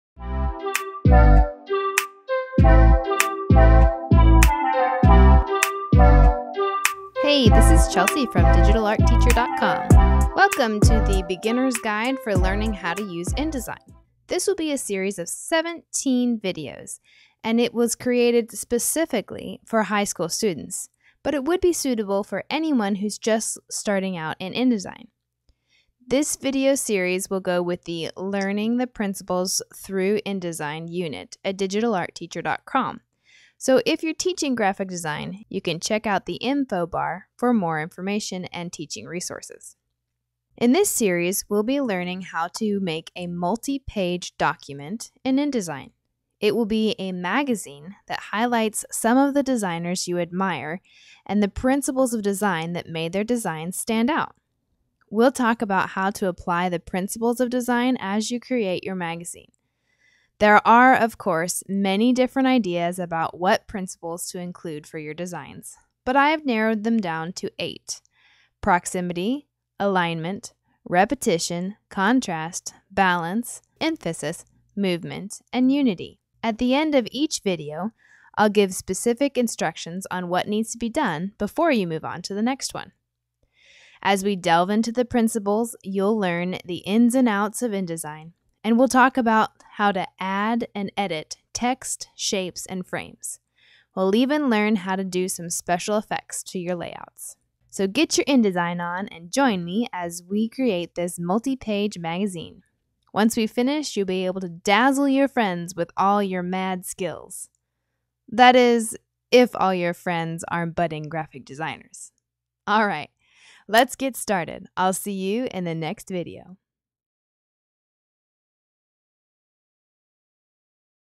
InDesign Beginner’s Tutorial: Learn to create a magazine
for lesson resources (including lesson plans, presentations, project outlines and more!).

This is an overview of what you can expect to learn in this 20-video series. In the series we’ll be creating a multipage document in InDesign and highlighting different principles of design.